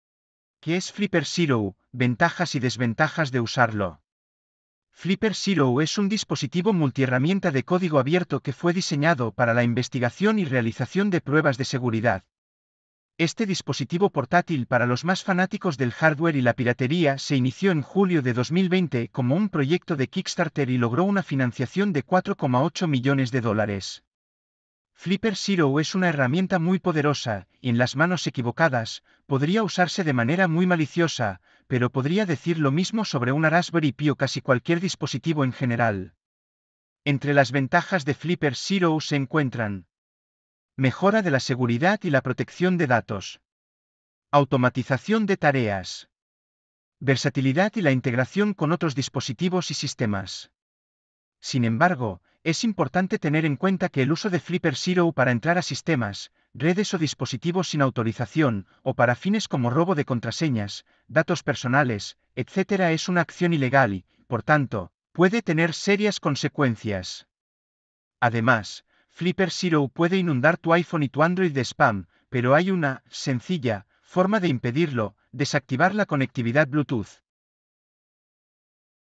Que es Flipper Zero Ventajas y desventajas de usarlo y como protegerse
Este video te muestra Que es Flipper Zero Ventajas y desventajas de usarlo y como protegerse